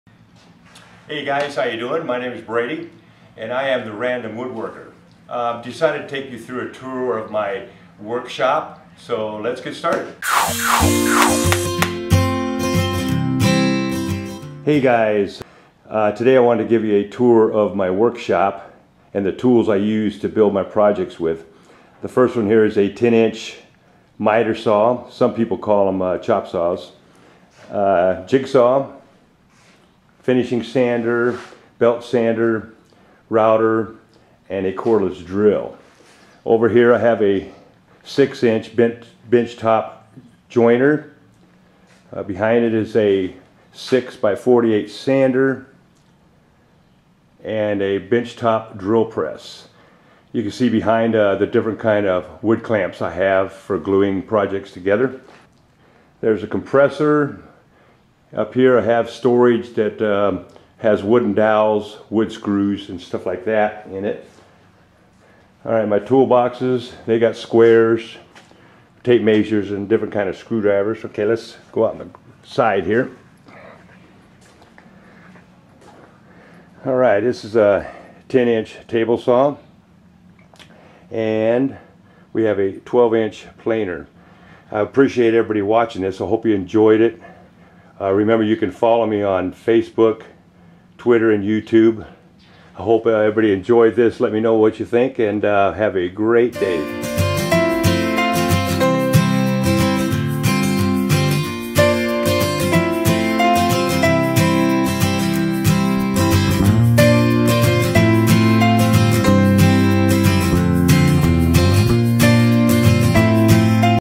Introduction - My Workshop Tour
Hey guys! I wanted to take some time to give you all the guided tour of my workshop where I build all of my woodworking projects and film all of my videos for the channel. I also give a quick run down of all the tools I use.